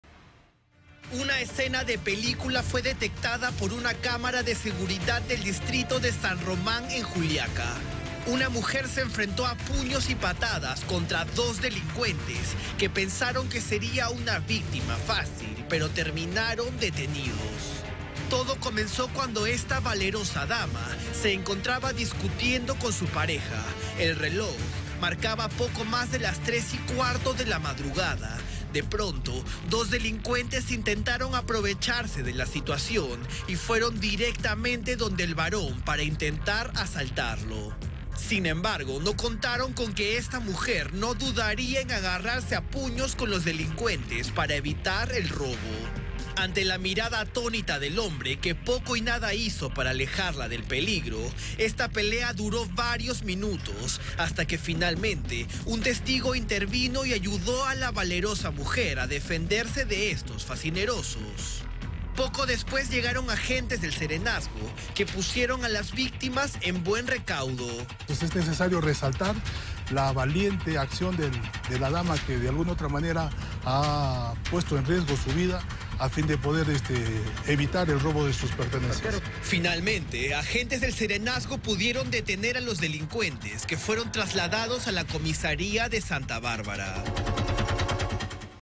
¡A golpes y patadas! mujer se enfrenta a delincuentes que intentaron asaltarla en Puno
En la ciudad de Puno, una mujer evitó ser una nueva víctima de la criminalidad, al enfrentarse a golpes y patadas contra dos delincuentes que intentaron asaltarla a ella y su pareja en horas de la noche.

Las cámaras de seguridad registraron el hecho. La fémina se encontraba discutiendo con su pareja, cuando los dos facinerosos los interceptaron e intentaron sacar provecho de la situación para despojarlos de sus pertenencias.

Rápidamente, la mujer se percata de sus intenciones e inició una pelea a puño limpio con los criminales, ante la mirada atónita de su pareja, que poco o nada hizo para alejarla del peligro.

El enfrentamiento duró por varios segundos, hasta que un testigo intervino para ayudar a la mujer. Casi al mismo tiempo, llegaron agentes del Serenazgo para poner a buen recaudo a la fémina y detener a los delincuentes, quienes fueron trasladados a la comisaría de Santa Bárbara.